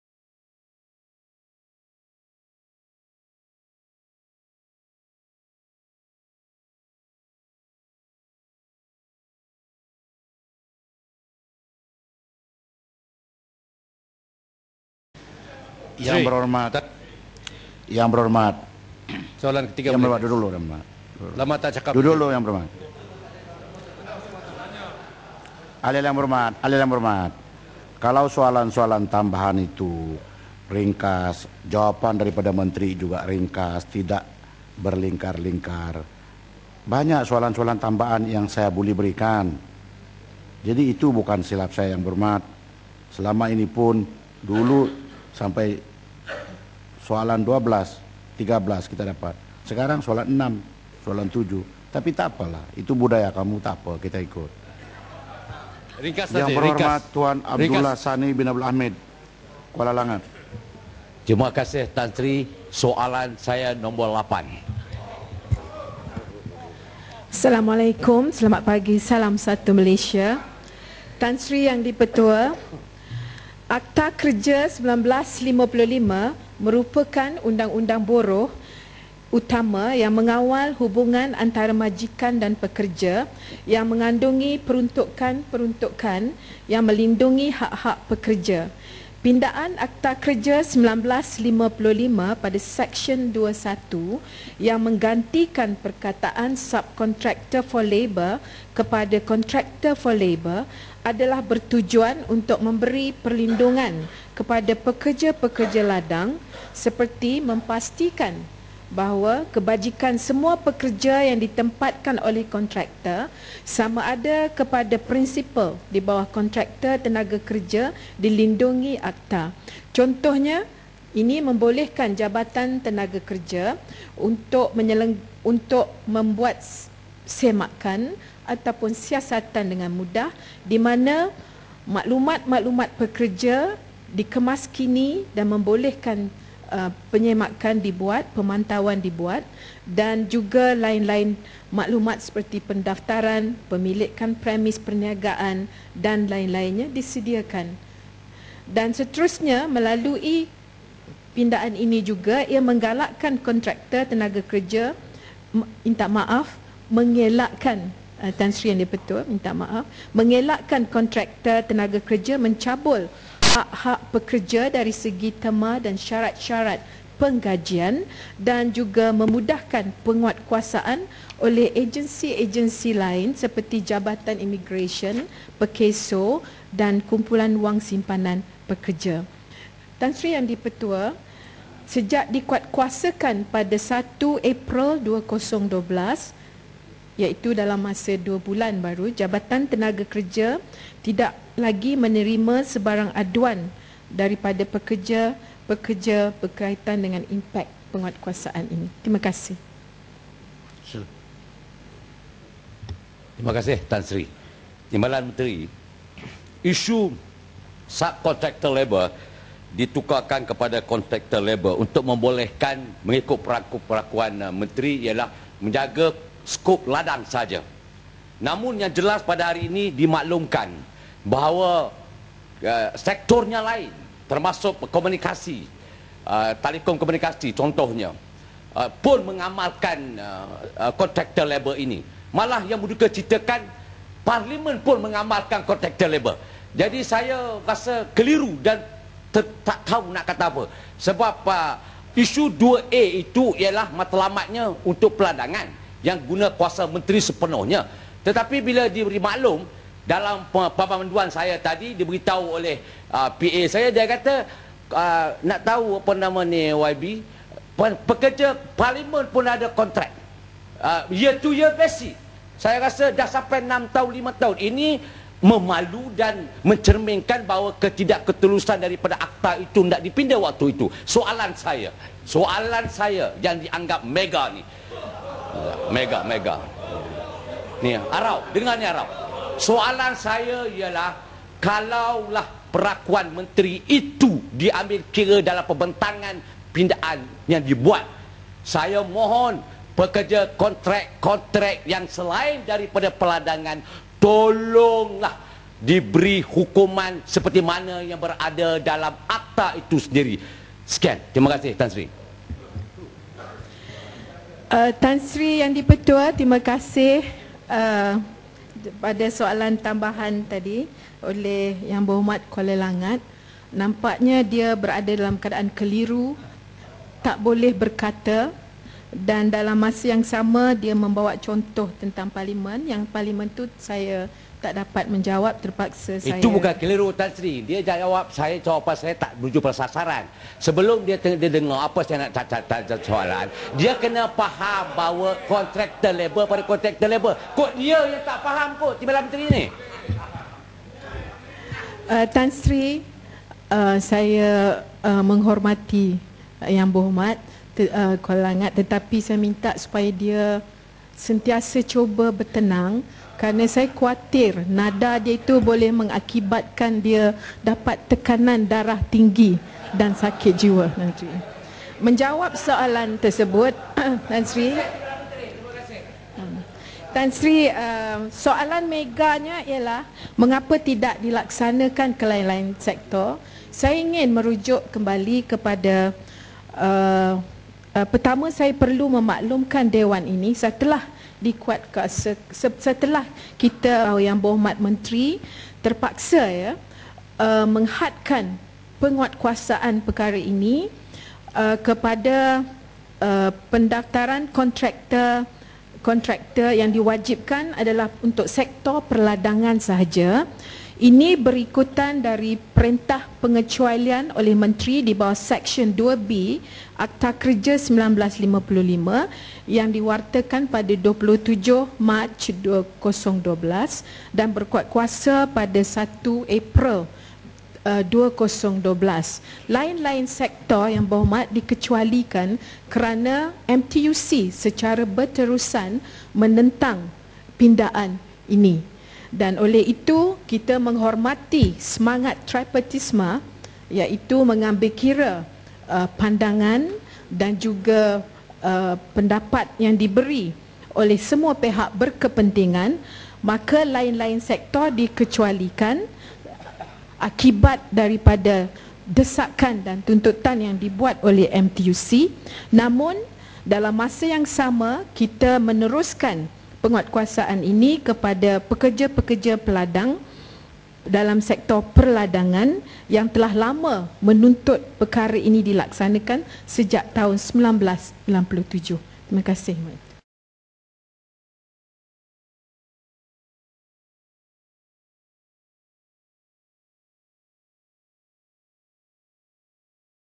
19 Jun 2012 - Sesi Soalan - Contractor Labour - PKR Kuala Langat
19 Jun 2012 - Sesi Soalan - Contractor Labour - PKR Kuala Langat

8. Tuan Abdullah Sani Abdul Hamid [Kuala Langat] minta Menteri Sumber Manusia
menyatakan keberkesanan pindaan 'subcontractor labour' kepada 'contractor labour' setakat ini dan kesan terhadap pekerja tempatan.

Tuan Abdullah Sani Abdul Hamid [Kuala Langat]: Terima kasih Tuan Yang di-Pertua.
Yang Berhormat Timbalan Menteri, isu subcontractor labour, ditukarkan kepada contractor labour untuk membolehkan mengikut perakuan Menteri ialah menjaga skop ladang sahaja. Namun yang jelas pada hari ini dimaklumkan bahawa sekornya lain termasuk telekomunikasi pun mengamalkan contractor labour ini. Malah yang mendukacitakan Parlimen pun mengamalkan contractor labour.
Jadi saya rasa keliru dan tidak tahu hendak kata apa sebab isu 2A itu ialah matlamatnya untuk perladangan yang guna kuasa Menteri sepenuhnya. Akan tetapi bila diberi maklum dalam pengaduan saya tadi diberitahu oleh PA saya, dia kata hendak tahu apa ini Yang Berhormat, pekerja Parlimen pun ada kontrak year-to-year basis.

Saya rasa sudah sampai enam tahun, lima tahun. Ini memalukan dan mencerminkan
ketidaktelusan daripada akta itu hendak dipinda waktu itu. Soalan saya, soalan saya yang dianggap mega ini... [Ketawa] Yang Berhormat Arau dengar ini. Soalan saya ialah kalaulah perakuan Menteri itu diambil kira dalam pembentangan pindaan yang dibuat, saya mohon pekerja kontrak-kontrak yang selain daripada perladangan tolonglah diberi hukuman seperti mana yang berada dalam akta itu sendiri. Sekian terima kasih.